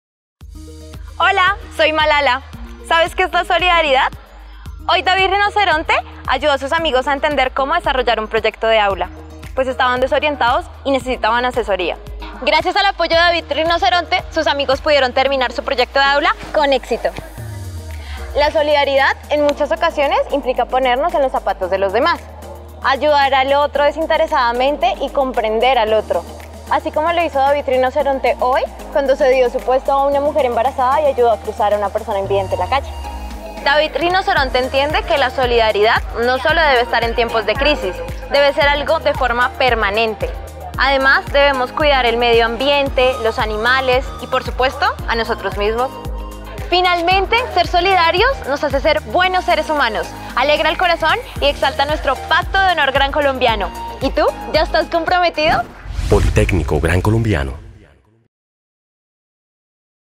David Rinoceronte, Pacto de Honor Grancolombiano
¿Sabes quién es David Rinoceronte?

Conoce más acerca de él y como ser parte del Pacto de Honor Grancolombiano.